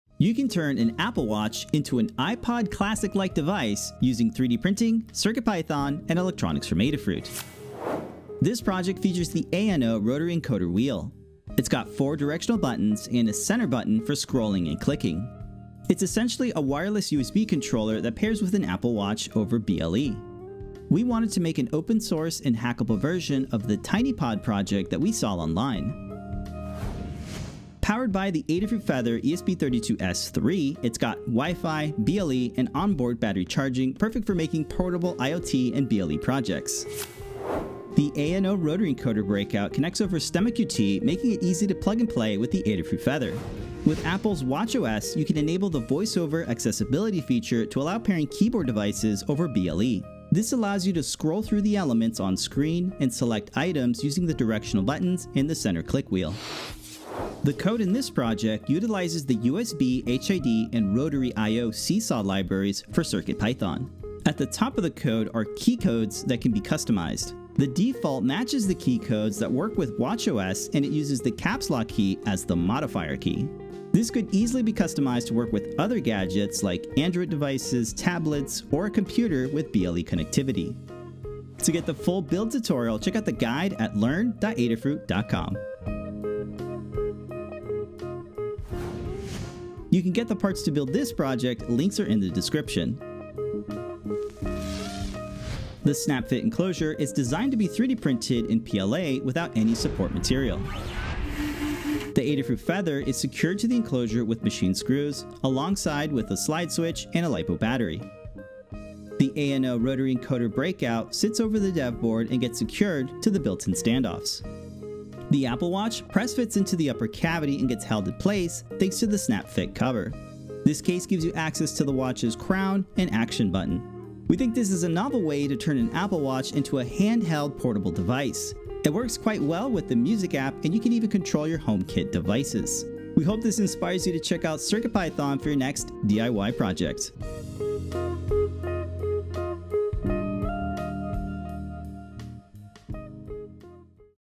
Turn an old Apple Watch into a mini iPod!
Turn an old Apple Watch into a mini iPod! adafruit 3dprinting Use BLE on a Feather ESP32 S3 running CircuitPython with an ANO rotary encoder wheel to control the watch interface